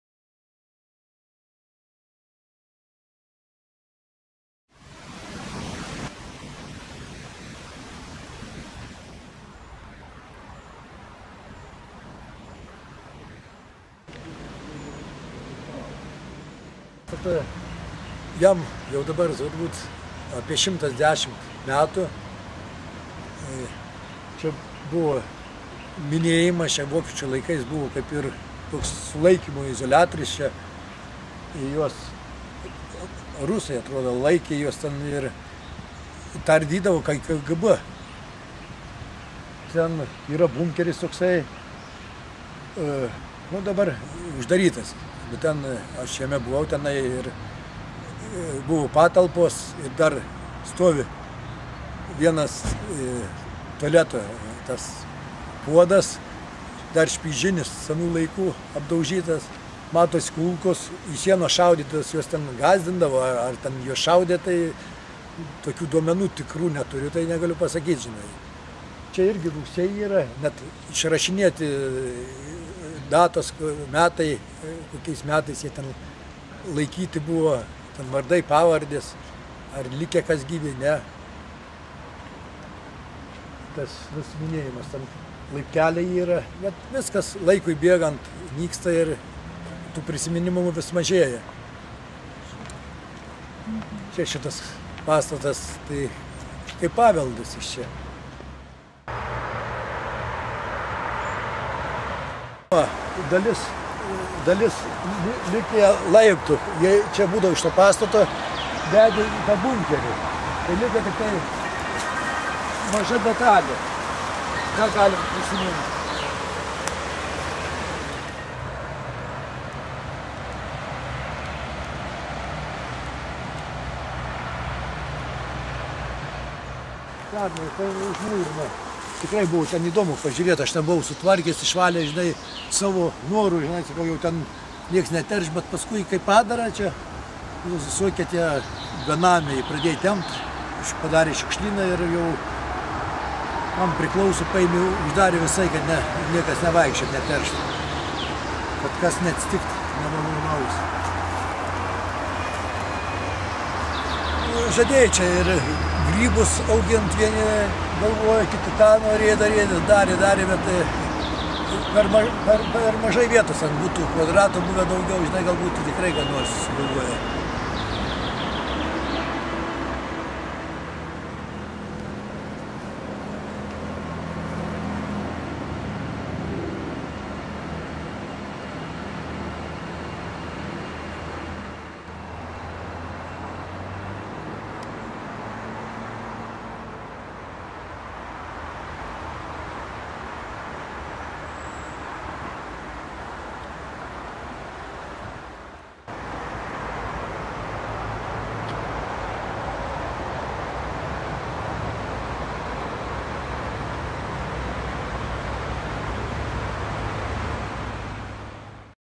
Paslaptinga Vytauto parko pusė
Pasakoja Vytauto parko darbuotojas Bernardas Šerpytis.
Įrašo autorė - projekto "Atminties vietos" tyrėja Rimantė Jaugaitė.